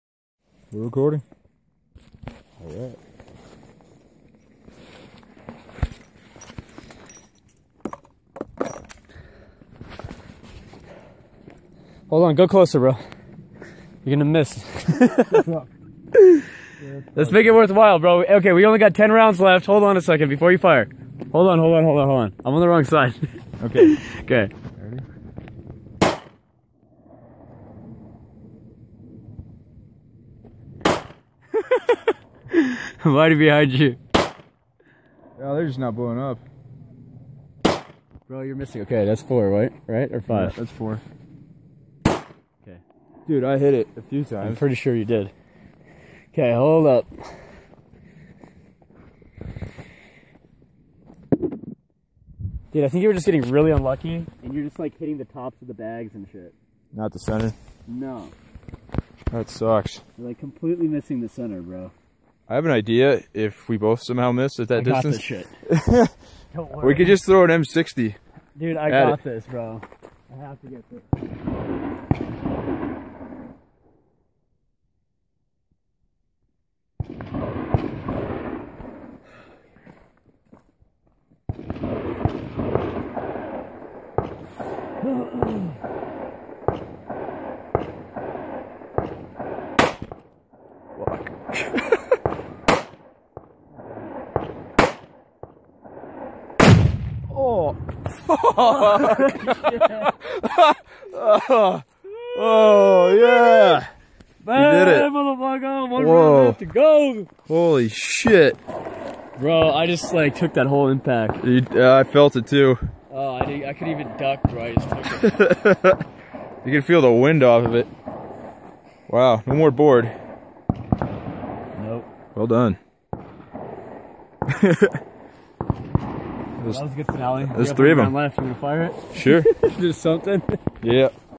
Shooting exploding targets in Las Vegas
Making things go boom